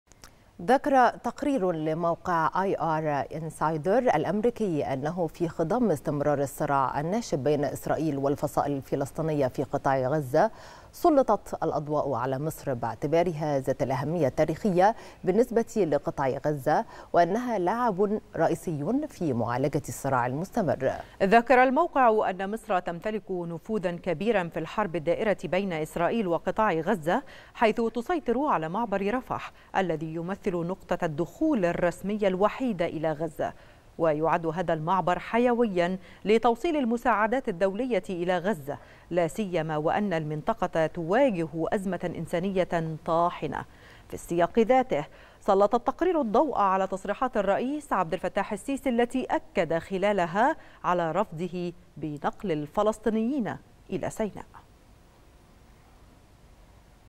تقرير أمريكي يسلط الضوء على دور مصر الدبلوماسي في معالجة الصراع بين إسرائيل والفصائل الفلسطينية
تقرير أمريكي يسلط الضوء على دور مصر الدبلوماسي والتاريخي في معالجة الصراع بين إسرائيل والفصائل الفلسطينية